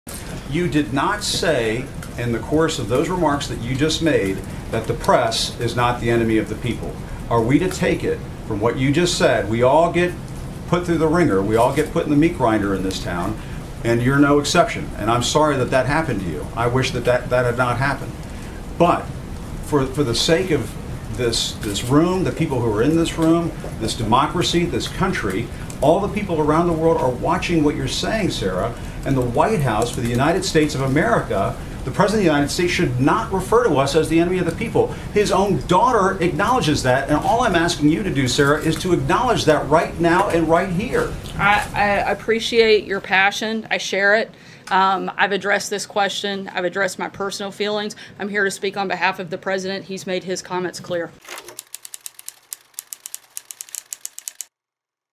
Sarah Huckabee Sanders Refuses To Say The Media Is Not The 'Enemy Of The People'
Sarah Huckabee Sanders was asked repeatedly by CNN reporter Jim Acosta about Ivanka Trump's comments on the media. Earlier, the first daughter had said that, unlike President Trump, she doesn't think the press is the enemy of the people.